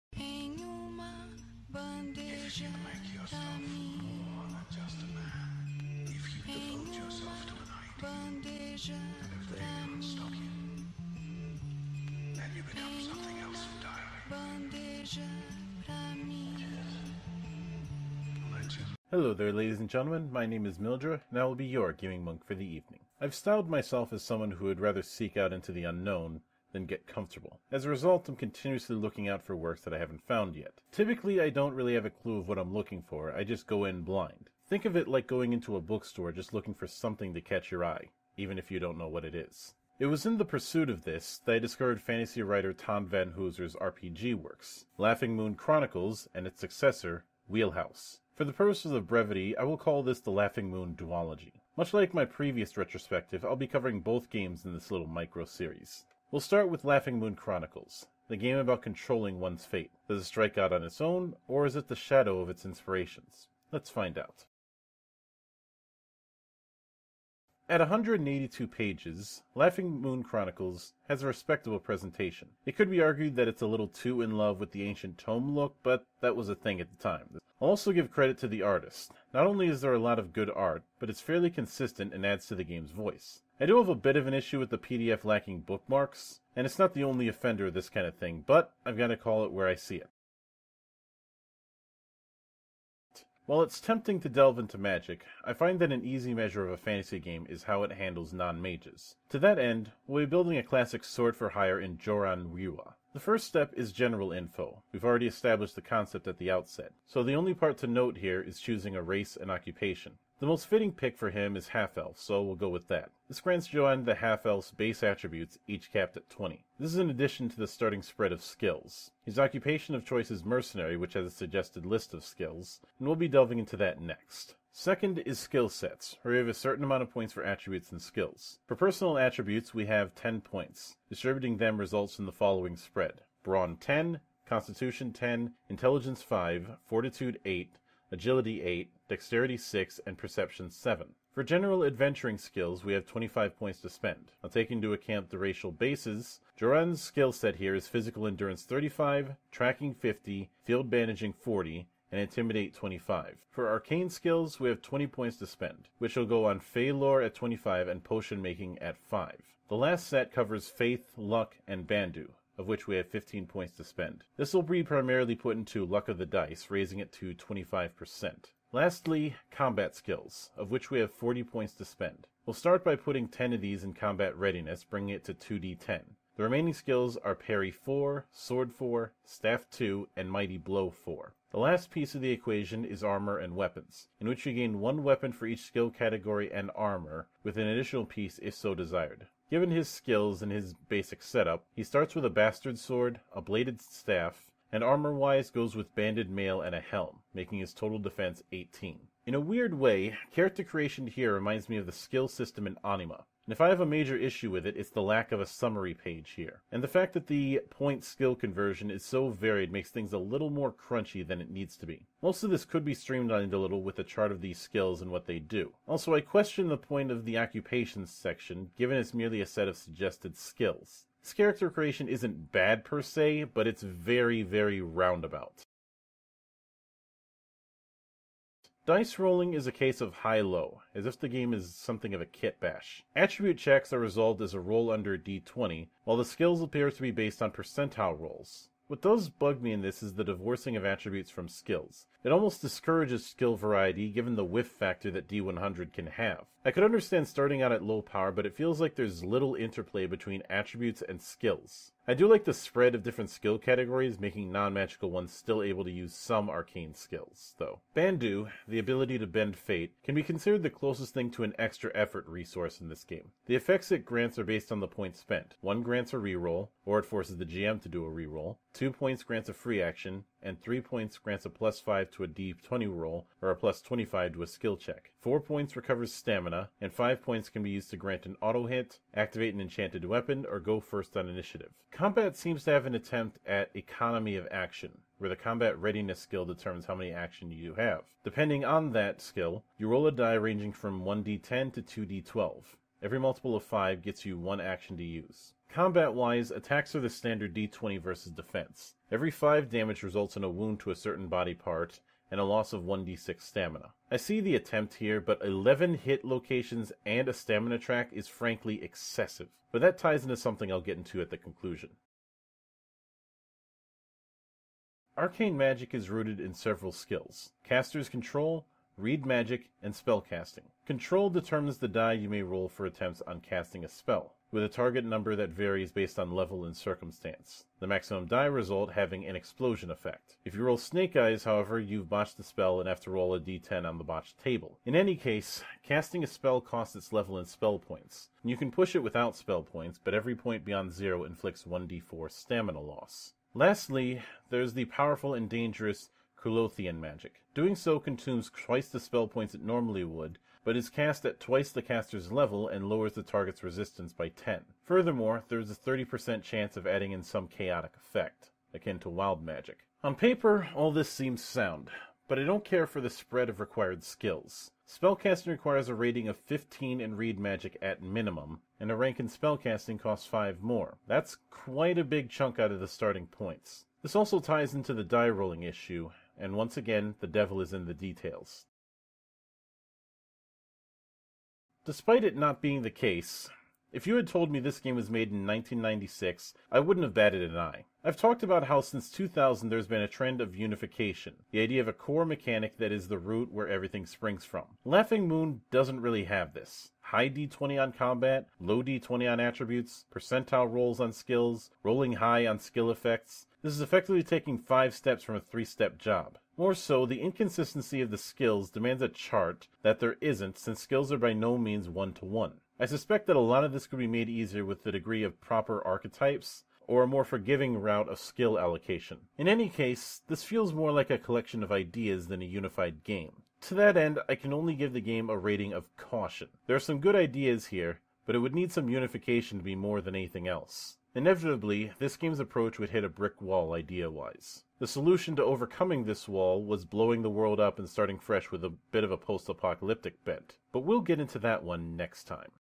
Gaming Monk Review #31: Laughing Moon Chronicles
Gaming Monk Review returns with a look at what happens when a novelist tries his hand at making a game. Part 1 of 2.